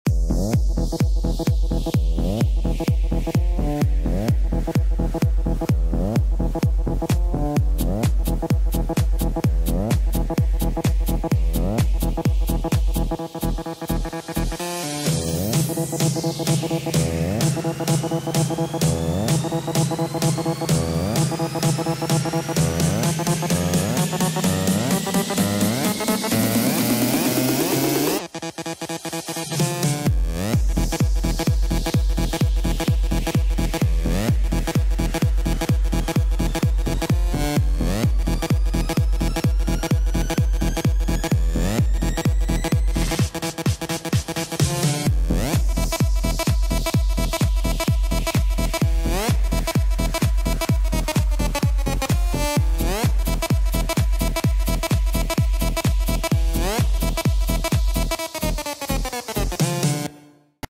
Wedding party in Armenia
We danced like crazy in Armenia !